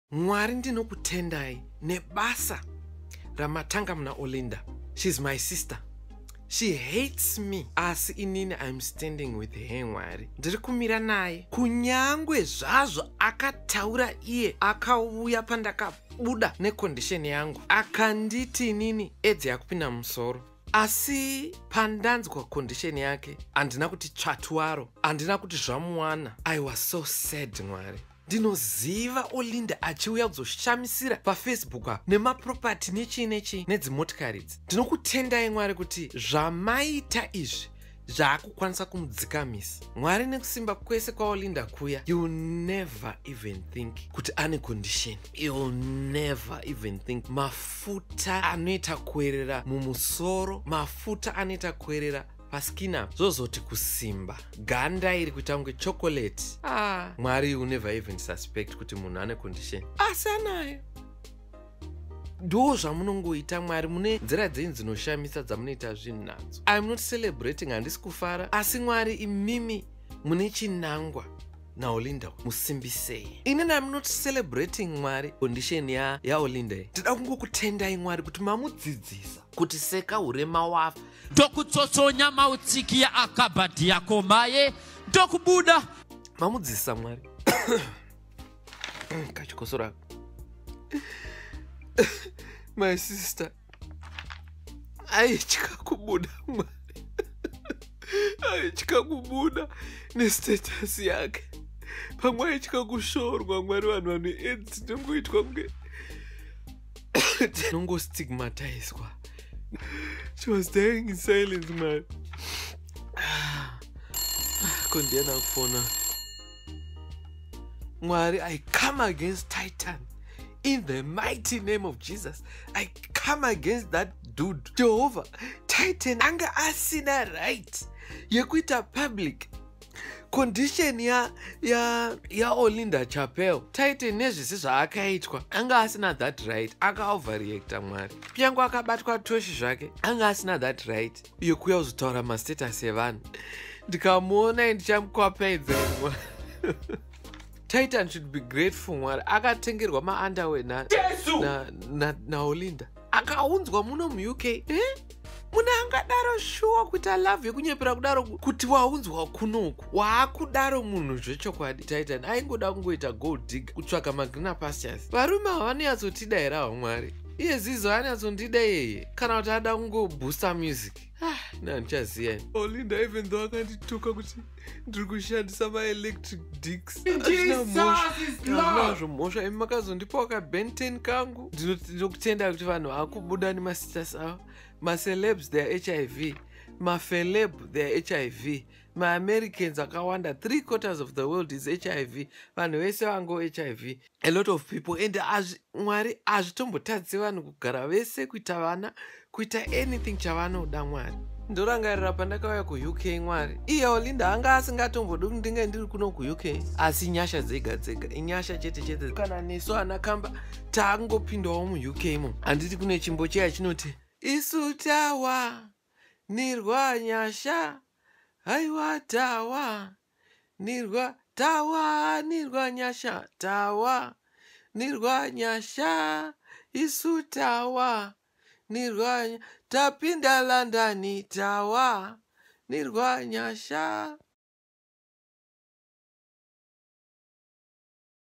Mai Titi's Prayer for Olinda After Tytan Comes Out With Her HIV Status
Recently MaiTiti did a live video where she was claimed standing with Olinda

Was Mai Titi genuinely standing with Olinda? You decide!